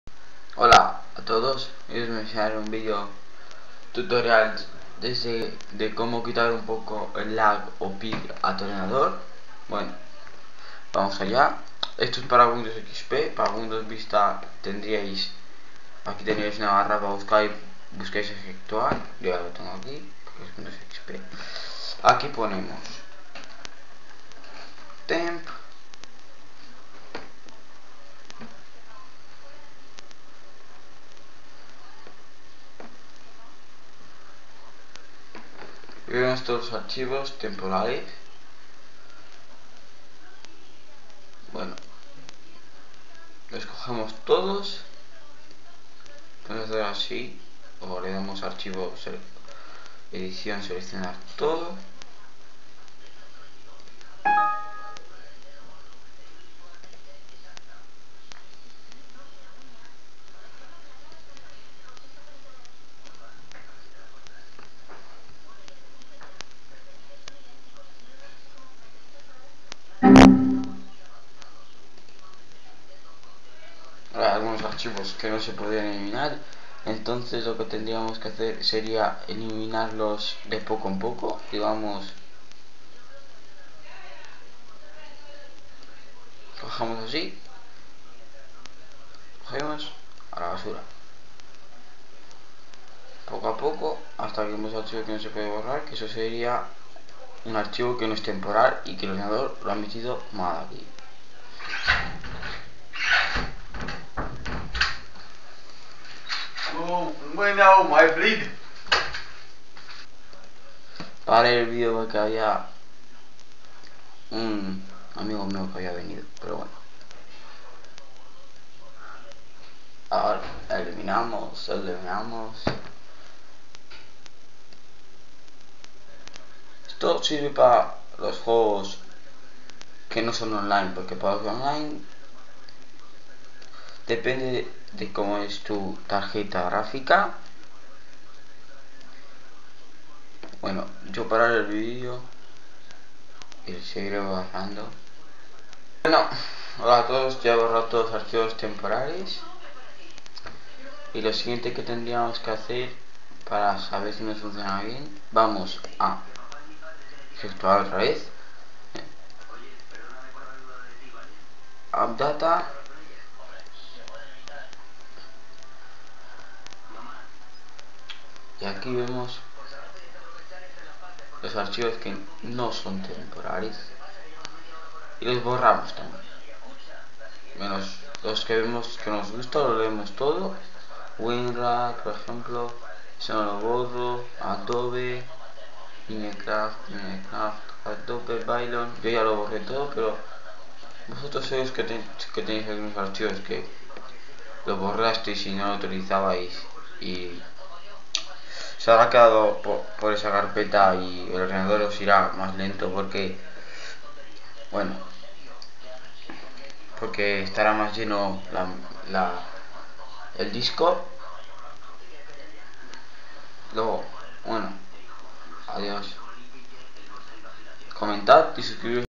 quitar lag y pig sin programas.
Esto es un video que me lo han pedido par aquitar el lag a sus juegos como al halo al counter strike ...